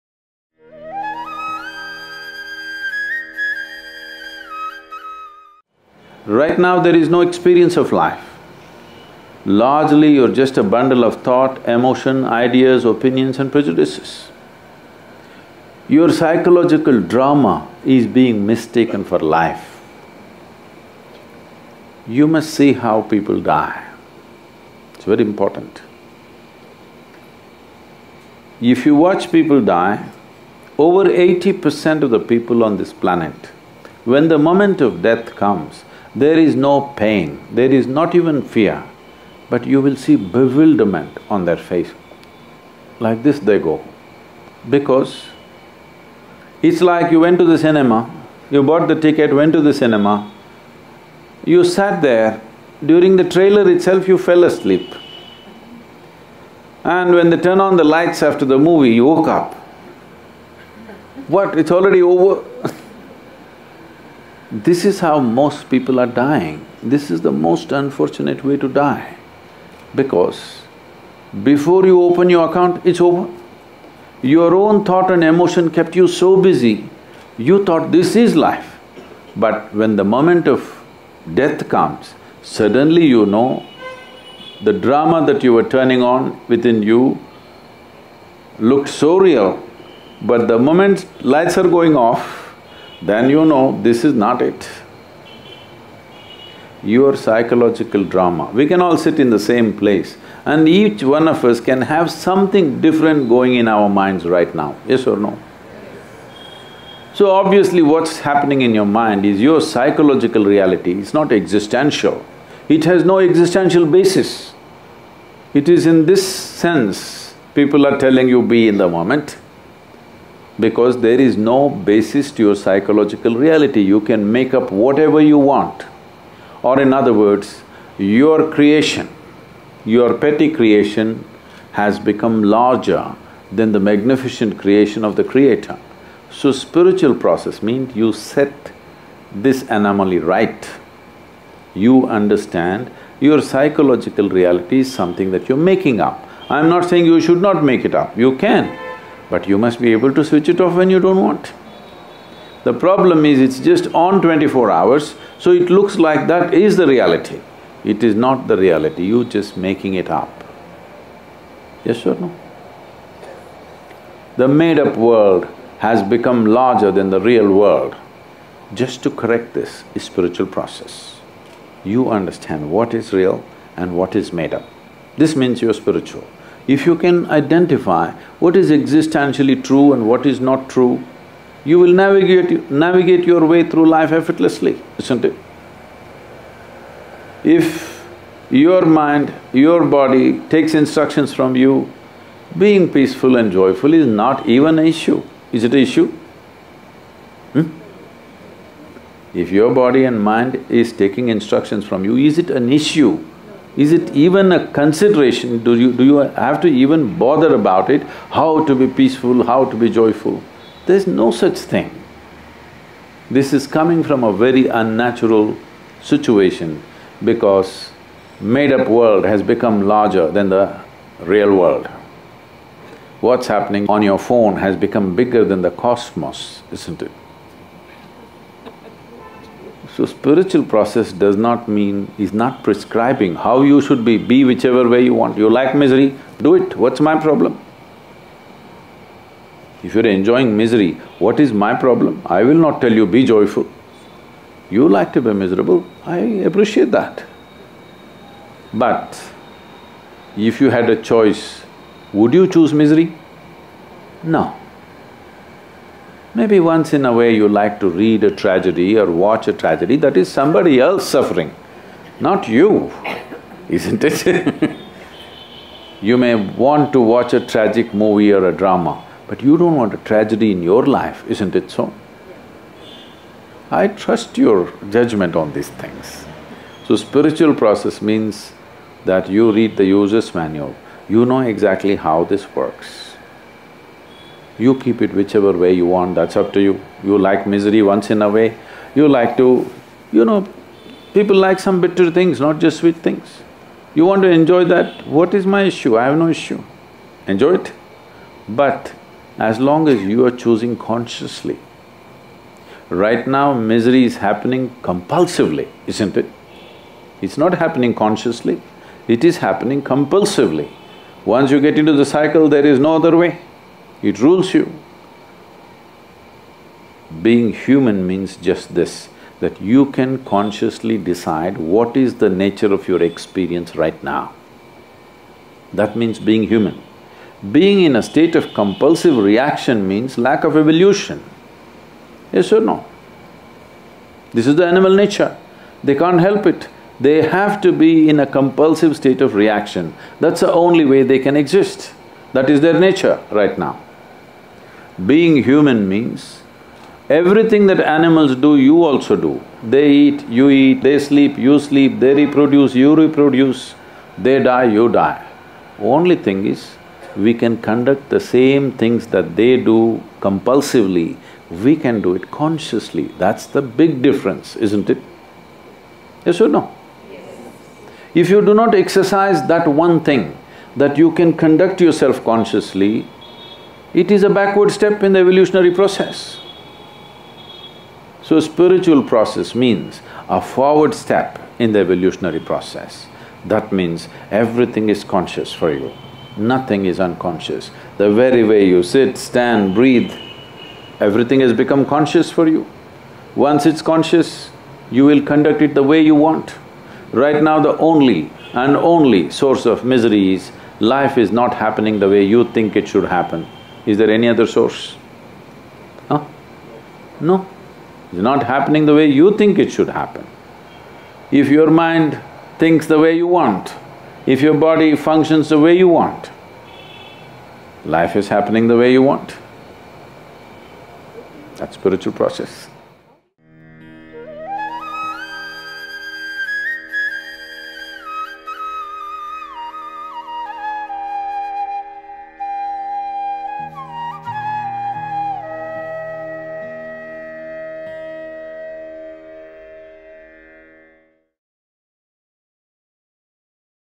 "Evolving into Consciousness" Sadhguru
Are you so engrossed in your phone screen that you are missing the cosmos? Sadhguru explains that for most of us, our psychological creation has become larger than the Creator’s magnificent creation. Correcting this compulsive behavior and evolving into conscious action is the essence of the spiritual process.

Sadhguru Talks @ An Evening with The Mystic, Pune, Jun 2017